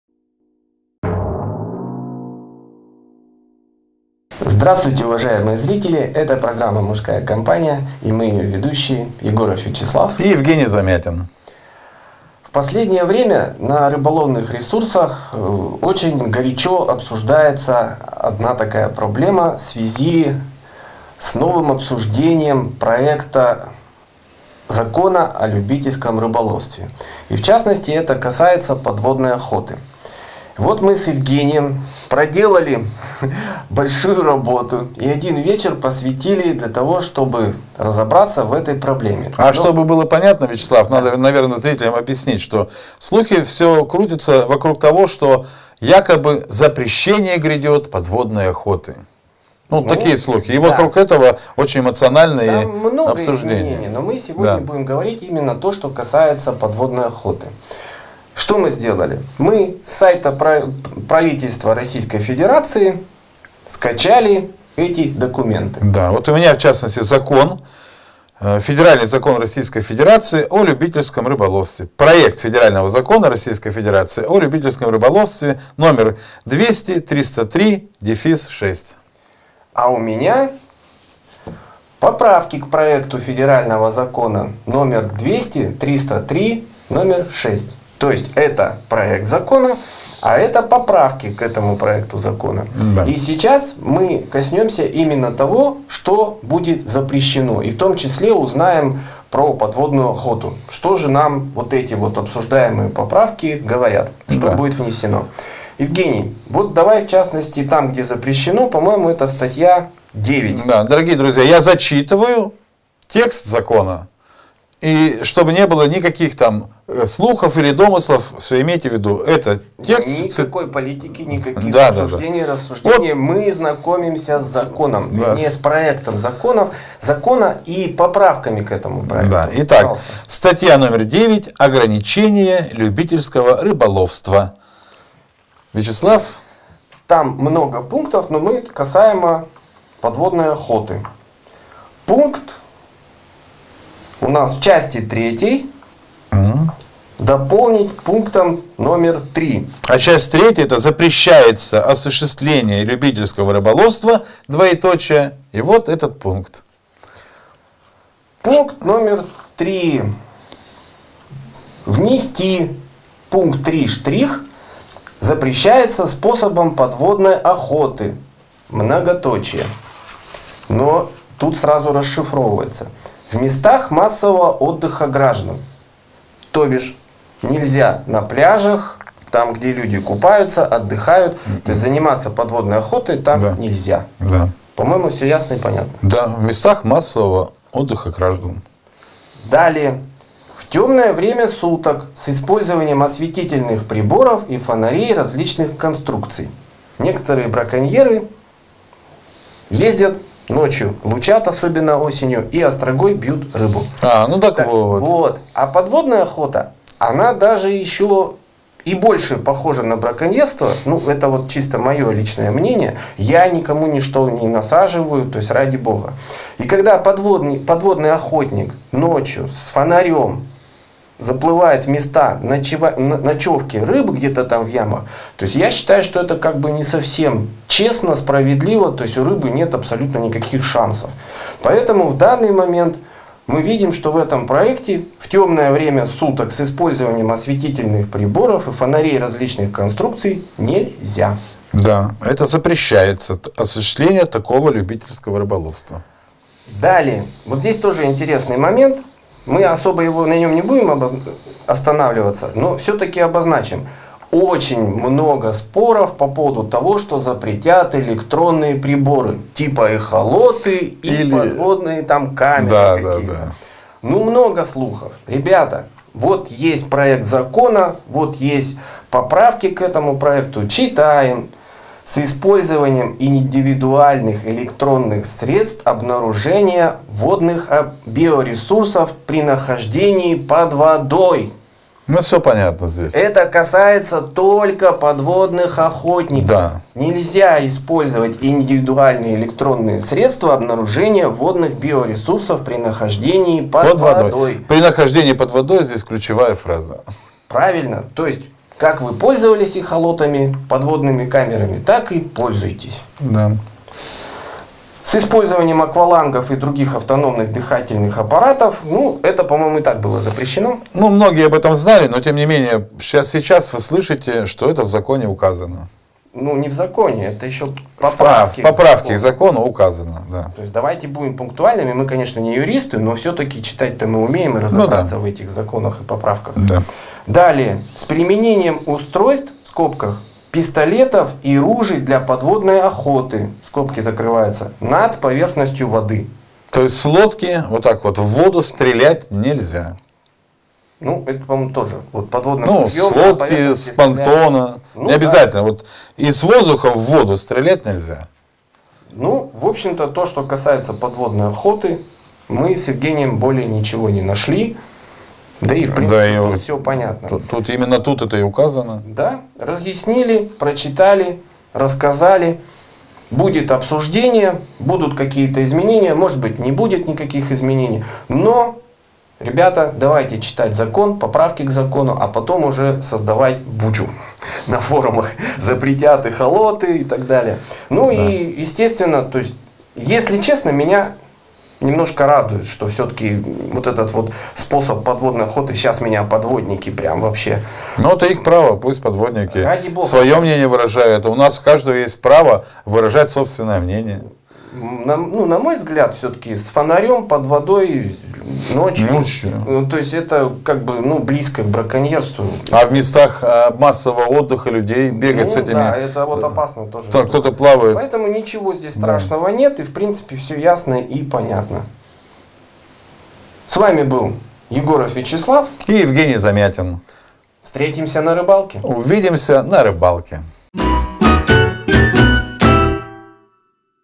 Законопроект О любительском рыболовстве Запрещается подводная охота ...
Законопроект О любительском рыболовстве. В статье 9:
1) часть 1 изложить в следующей редакции:
"1. В целях сохранения и рационального использования водных биоресурсов и среды их обитания устанавливаются ограничения любительского рыболовства:
1) запрет добычи (вылова) водных биоресурсов в определенных районах и в отношении отдельных видов водных биоресурсов;
2) минимальные размер и вес добываемых (вылавливаемых) водных биоресурсов;
3) виды и количество разрешаемых орудий, устройств (пистолетов и ружей для подводной охоты) и способов добычи (вылова) водных биоресурсов;
4) размер ячеи орудий добычи (вылова) водных биоресурсов, размер и конструкция орудий добычи (вылова) водных биоресурсов;
5) периоды добычи (вылова) водных биоресурсов в водных объектах рыбохозяйственного значения.";
2) в части 2 слова "объектов любительского рыболовства" заменить словами "водных биоресурсов";
3) в части 3: в пункте 2 слова "случаев, определенных правилами любительского рыболовства" заменить словами "случая, установленного частью 7 настоящей статьи";
дополнить пунктом 3 следующего содержания:
"3) способом подводной охоты:
в местах массового отдыха граждан;
в темное время суток с использованием осветительных приборов и
фонарей различных конструкций;
с использованием индивидуальных электронных средств обнаружения
водных биоресурсов при нахождении под водой;
с использованием аквалангов и других автономных дыхательных
аппаратов;
с применением устройств (пистолетов и ружей для подводной охоты) над
поверхностью воды.";